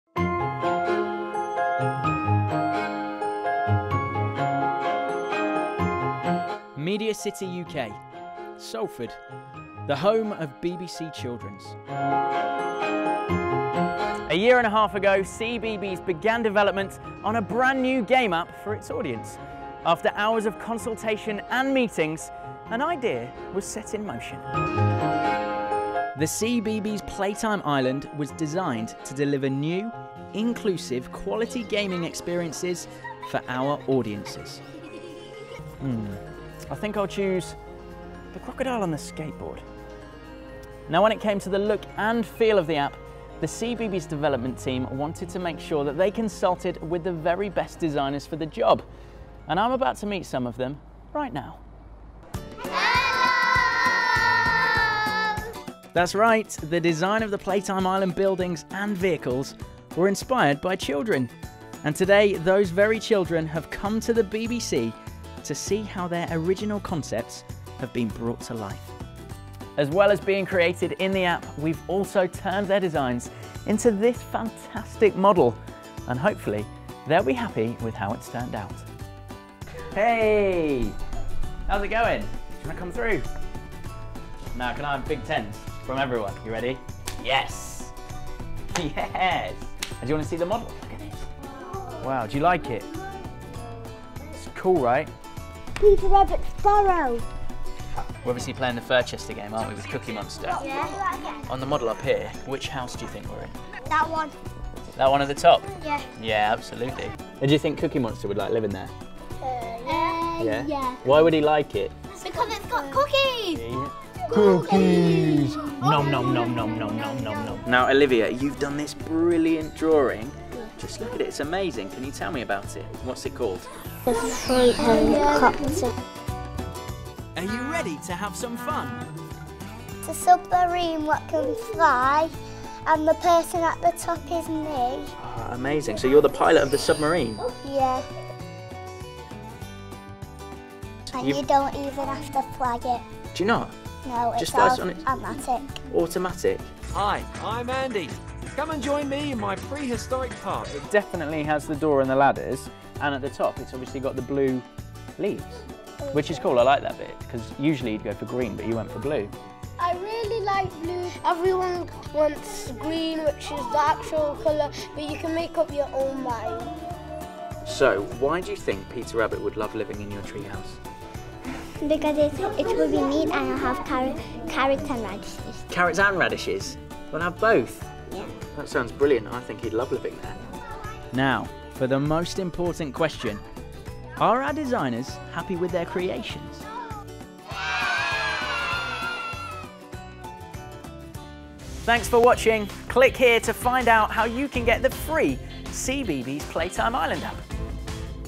CBeebies | Meet The Playtime Island App Designers!
Join Ben from the CBeebies House as he meet's the designers behind the new CBeebies Playtime Island App!

Grown-ups, find out how to get the brand new CBeebies Playtime Island app